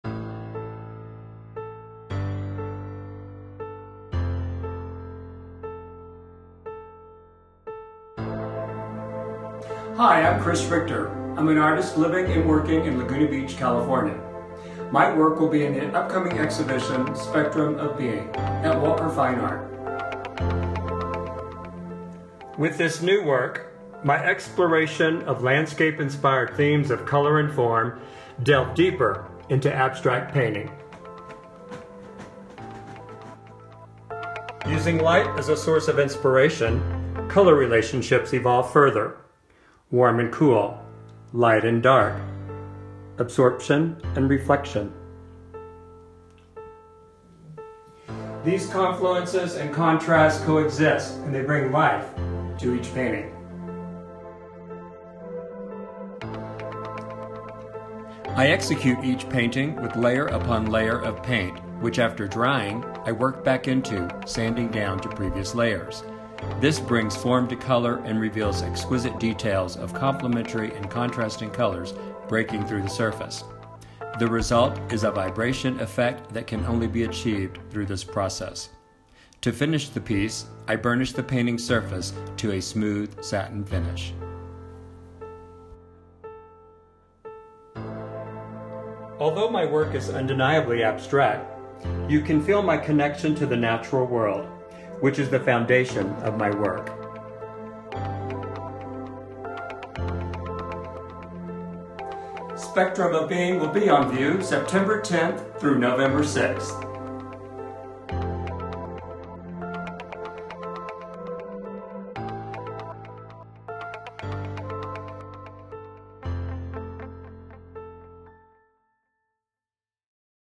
Artist Spotlight: Chris Richter
Hailing from Laguna Beach, CA, Richter looks to landscape, color and form for inspiration. Using light as an additional source for inspiration, color relationships evolve further: warm and cool, light and dark, absorption and reflection.
Contrasts & confluences coexists, bringing life to each painting. Richter begins his process with layer upon layer of paint, which after drying, he works through, sanding down to previous layers, revealing exquisite details achieved only through this process.
Chris Richter is a featured artist in our upcoming exhibition SPECTRUM OF BEING, opening Friday, September 10 from 5-8 PM.